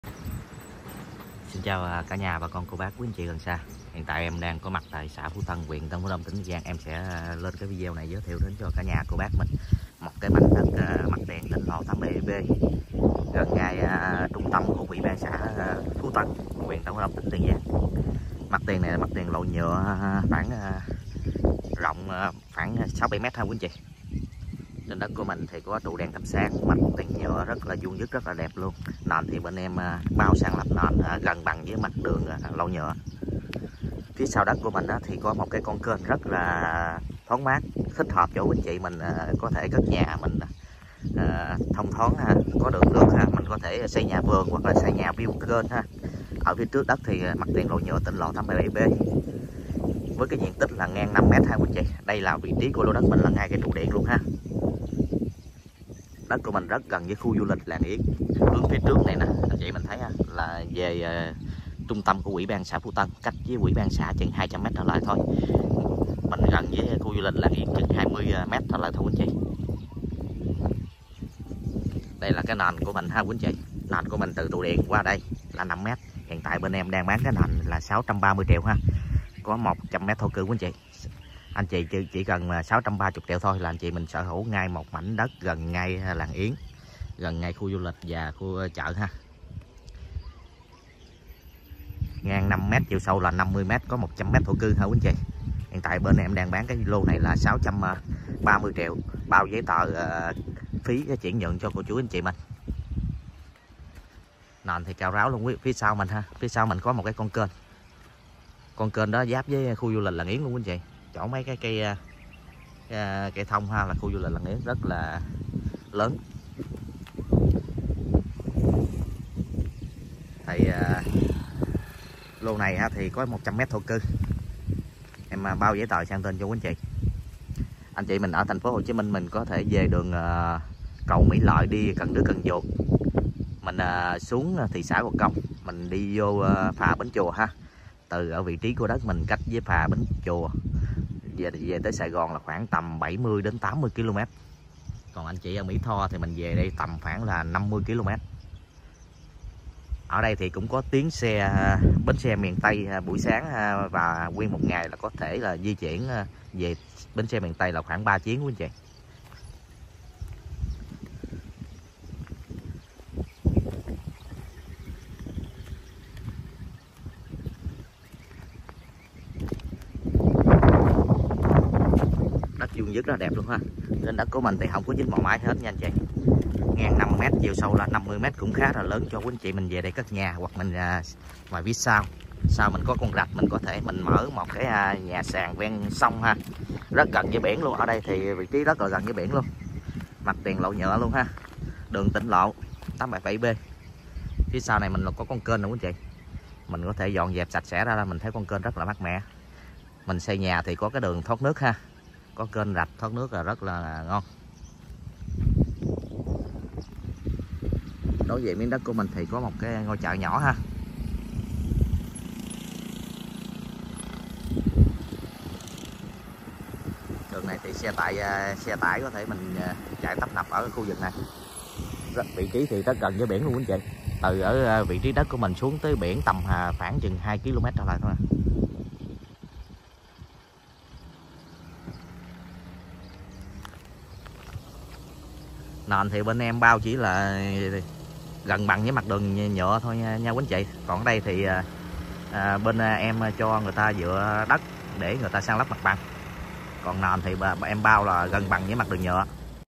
MS1357: 🌴Đã bán 🌴Đất tỉnh lộ 877B ngay chợ, ngay khu du lịch giá 630 triệu ( 5mx50m thổ cư)
Bán đất thổ cư tỉnh lộ 877B ngay khi du lịch Làng Yến
Đối diện chợ mới Phú Tân
Nền cao ráo sẵn
Tách thửa ngang 5m dài 50m có 100m2 thổ cư
Dân cư hiện hữu liền kề
Cách ủy ban 100m
Địa chỉ: Phú Tân, Tân Phú Đông, Tiền Giang